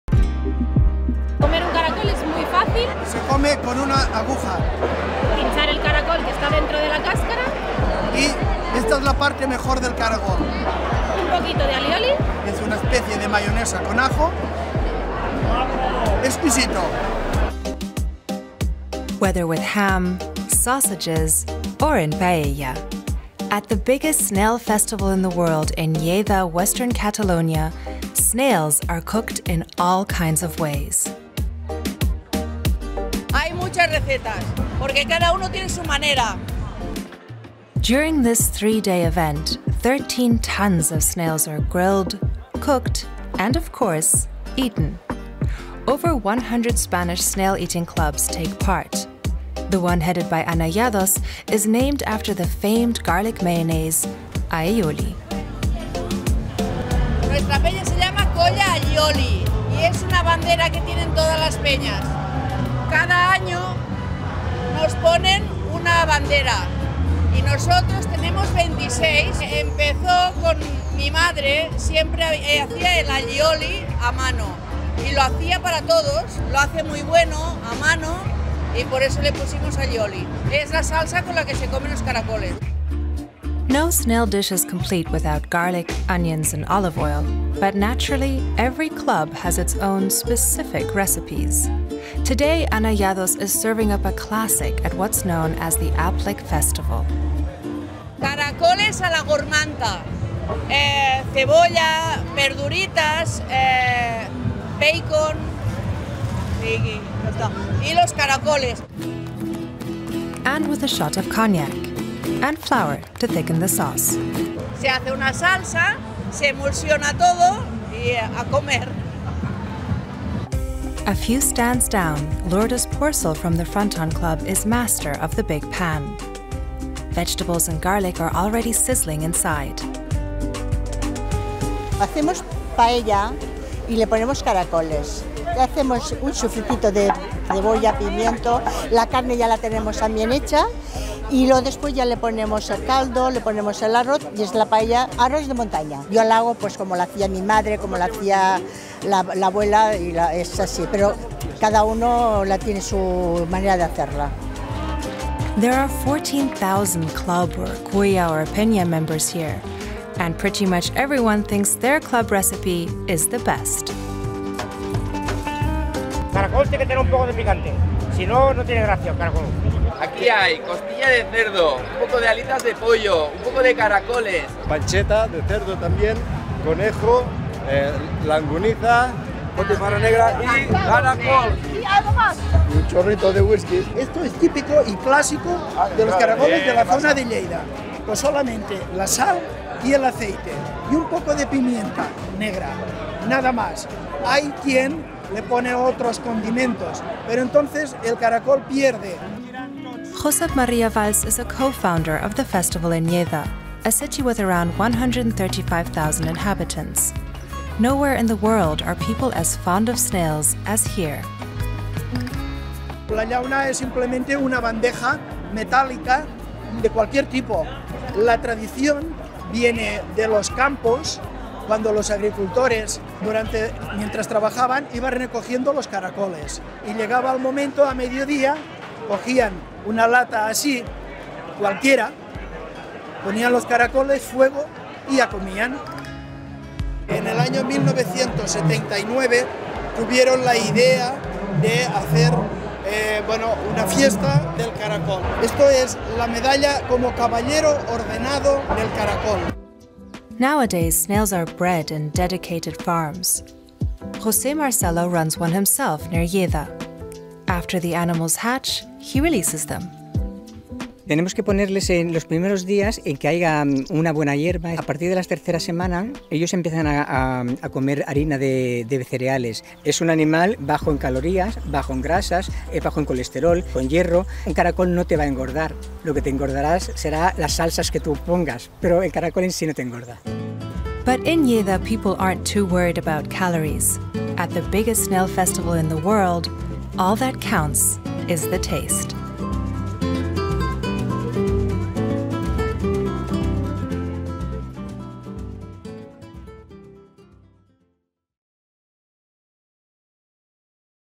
Delicious Or Disgusting? Spain's Biggest Snail-Eating Festival
In Lleida, 160 km north of Barcelona, there's a culinary weekend festival devoted entirely to the humble snail. Three days, around a million snails, and 200,000 snail gourmands – and then endless grilled snails, boiled snails, snail stew, snail rice, snails with sausage or other meat... 102 snail clubs or 'penyas' gather together at the festival site to sample the shelled gastropods. Lleida's snail festival is an age-old tradition, and was revived after the Franco dictatorship. In the past, the slimy creatures – known locally as 'caracol' – were considered peasant fare, but today they're viewed as a delicacy.

CREDITS
Report: Regina Niedenzu
Camera & Edit: Dirk Rensmann